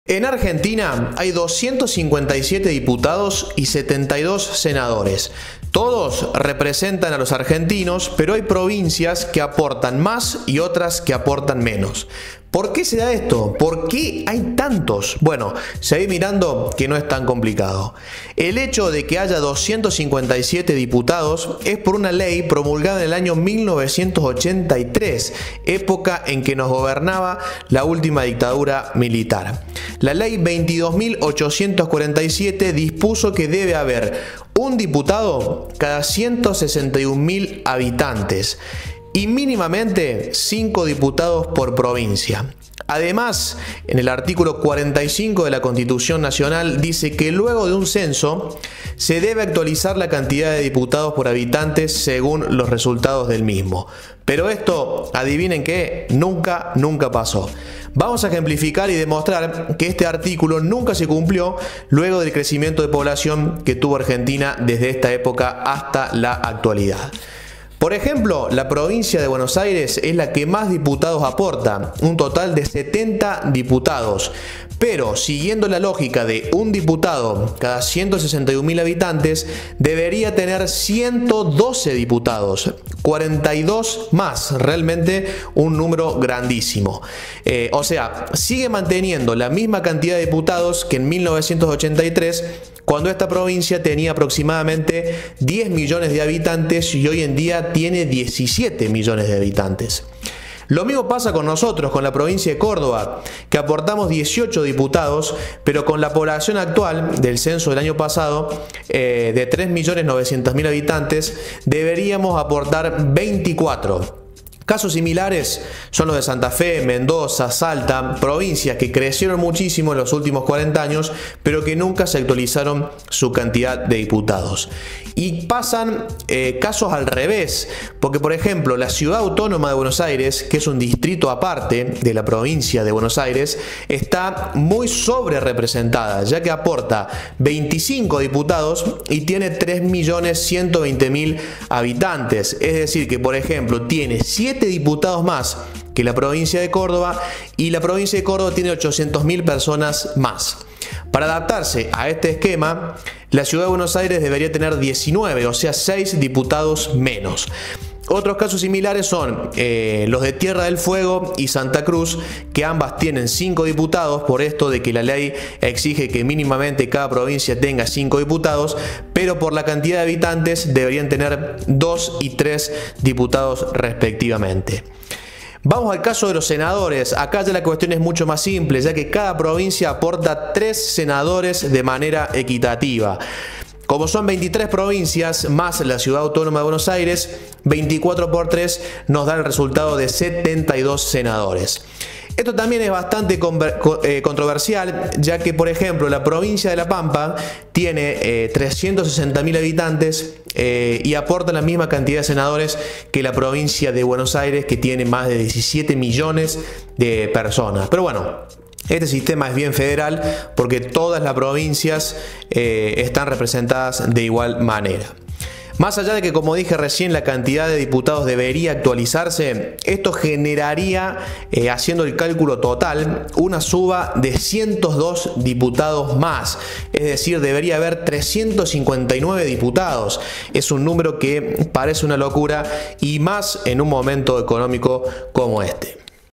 ¿Por qué hay tantos DIPUTADOS Y SENADORES en Argentina?
Cual es el sistema de reparto de diputados por provincia dentro de la Argentina?